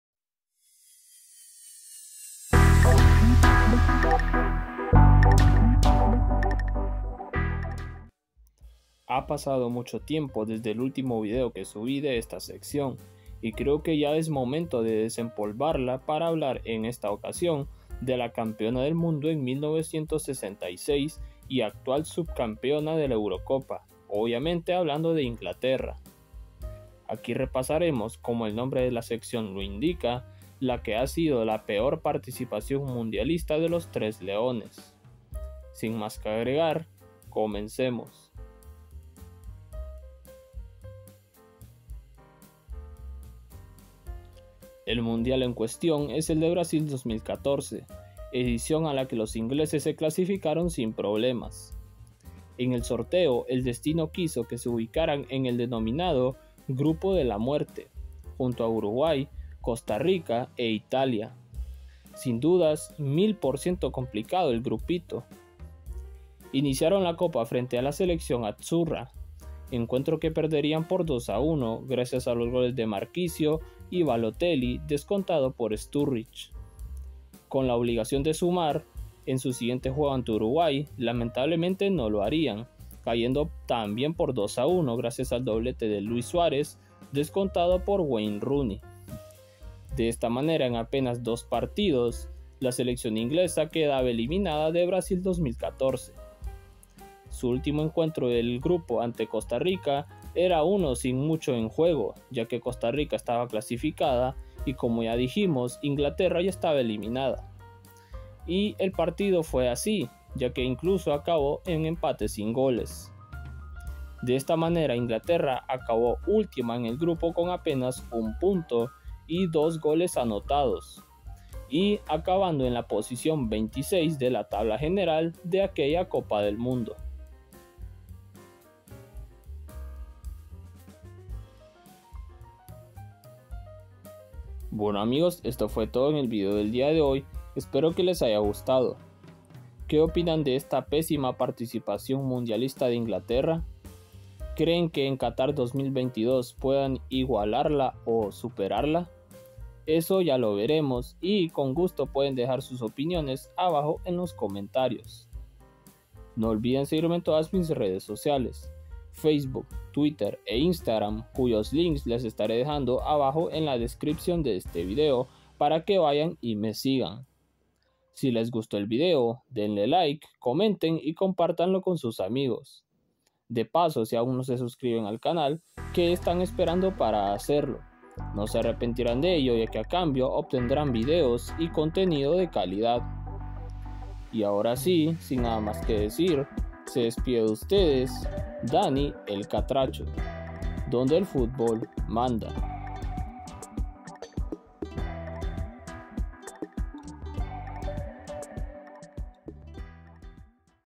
LA PEOR PARTICIPACIÓN MUNDIALISTA DE: Inglaterra 🏴󠁧󠁢󠁥󠁮󠁧󠁿 | Dani El Catracho
En el regreso de esta sección del canal, veremos la que ha sido la peor participación mundialista de la selección de Inglaterra 🏴󠁧󠁢󠁥󠁮󠁧󠁿